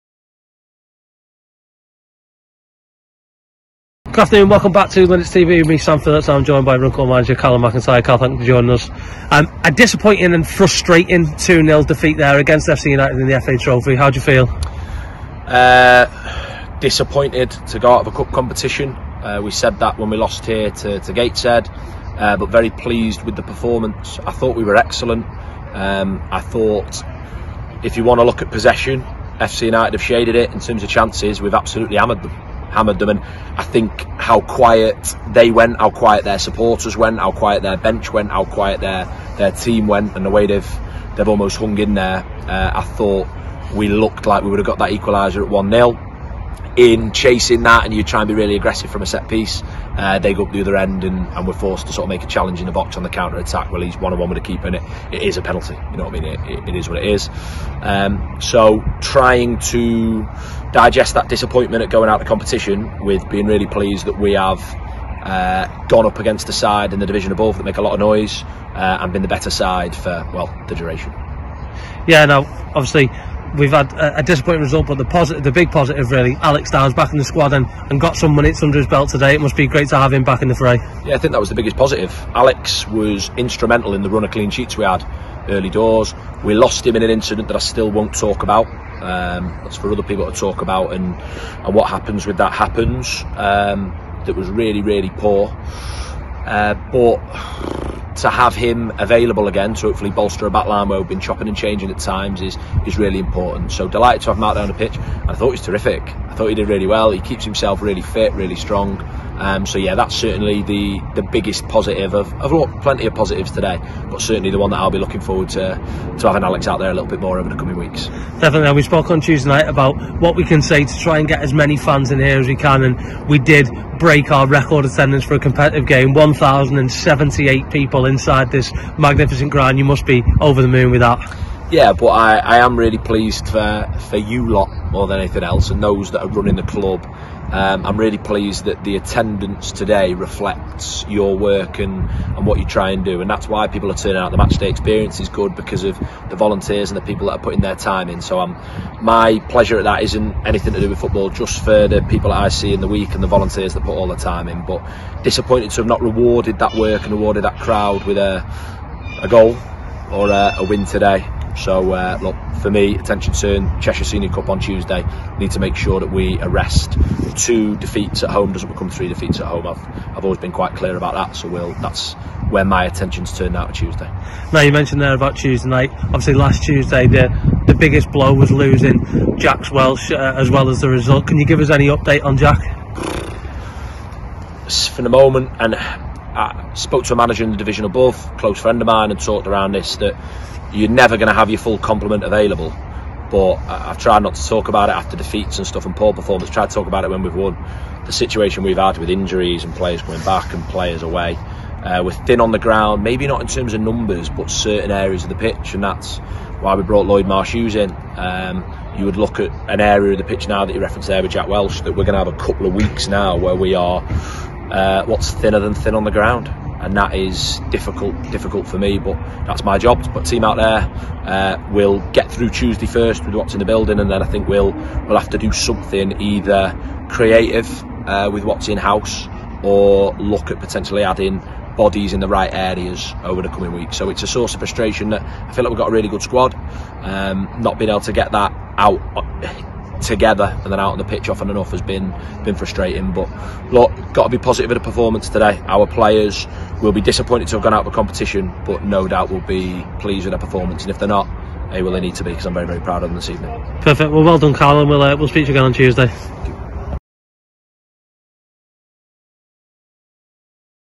Calum McIntyre Post Match Interview VS FC United of Manchester
🟡🟢 | Runcorn Linnets manager Calum McIntyre joined Linnets TV's Sam Phillips after today's frustrating 2-0 defeat to FCUM in the FA Trophy. Calum discusses the game, the return of Alex Downes and how happy he was with the attendance record being broken.